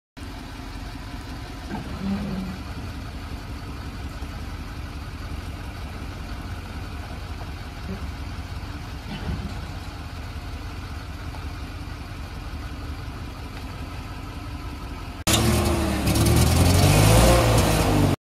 GovDeals:2007 Chevrolet C5500 4x4 Service Truck
2007 Chevrolet C5500 4x4 Service Truck sold on GovDeals.com by Liquidity Services - CA